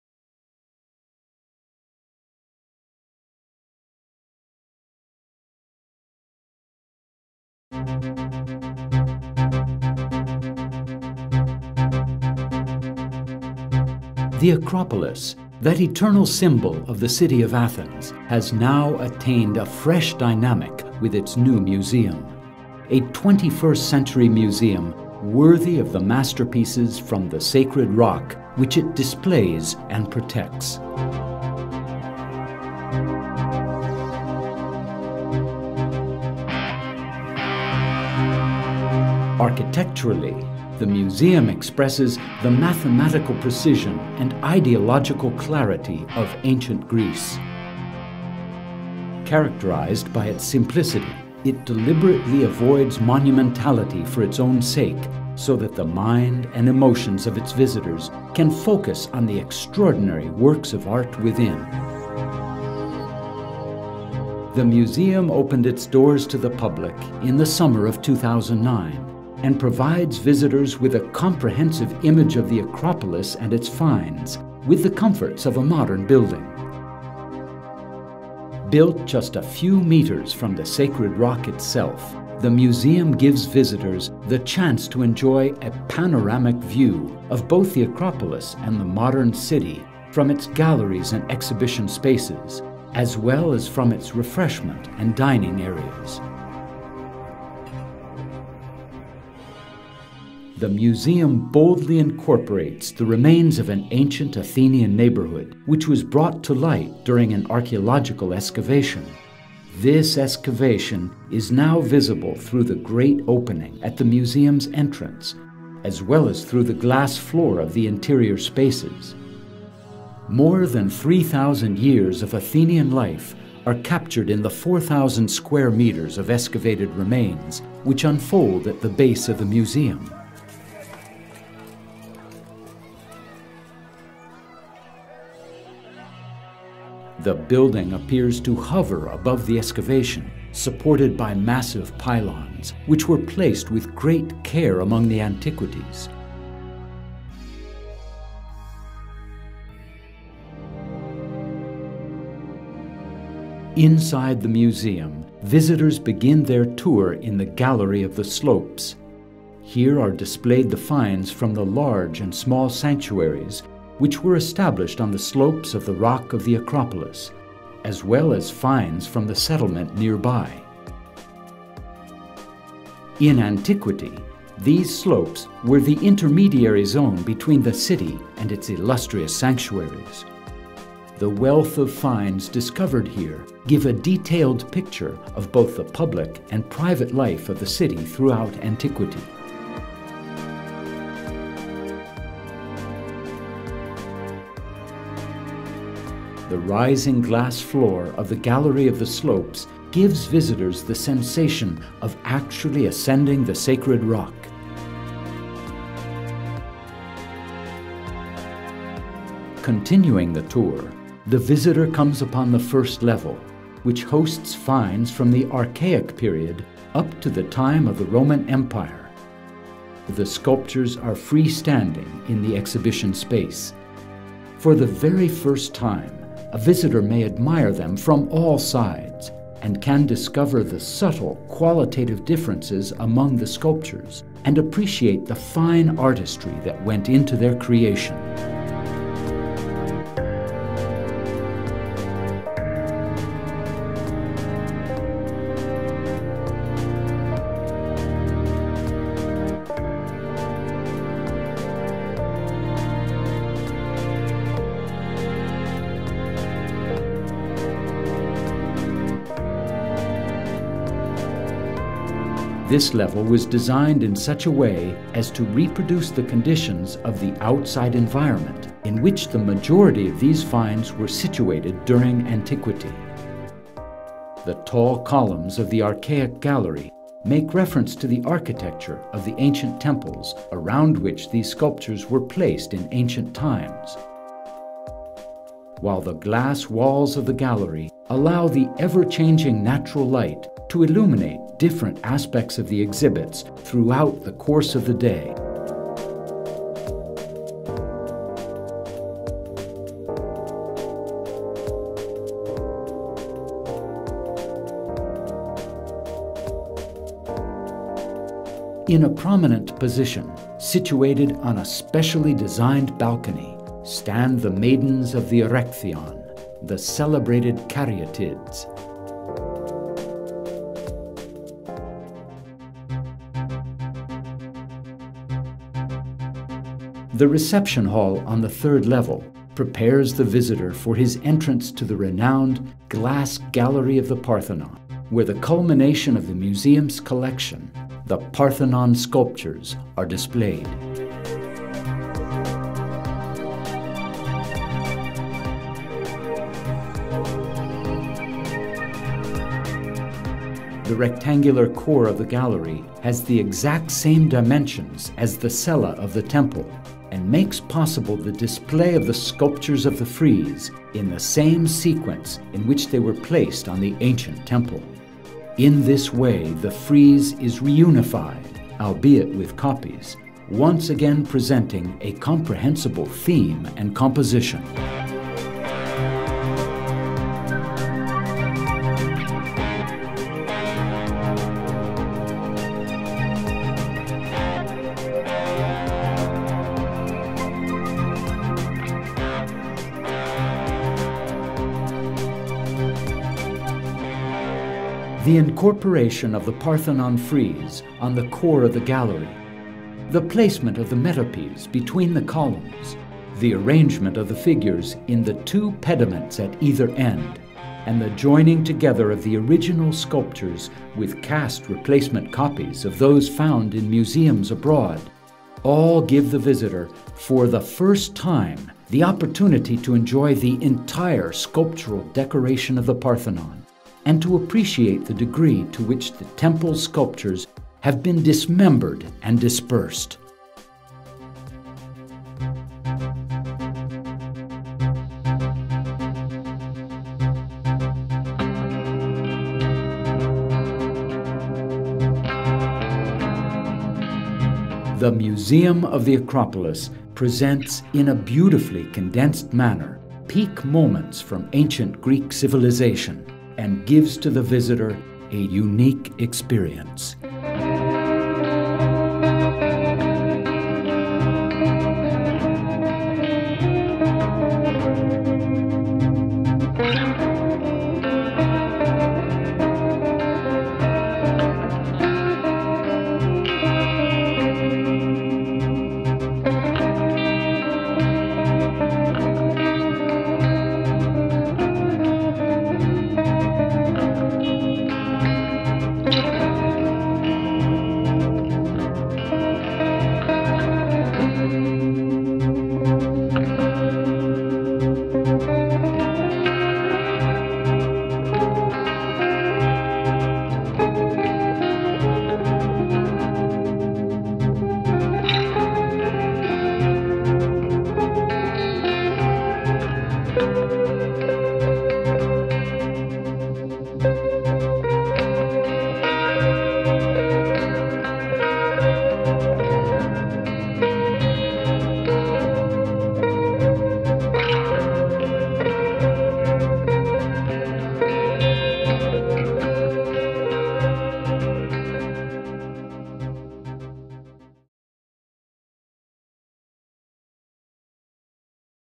Bảo tàng Acropolis - Bernard Tschumi Architects
Video giới thiệu về 1 trong những bảo tàng độc đáo nhất thế giới.
Bản quyền video thuộc Acropolis Museum.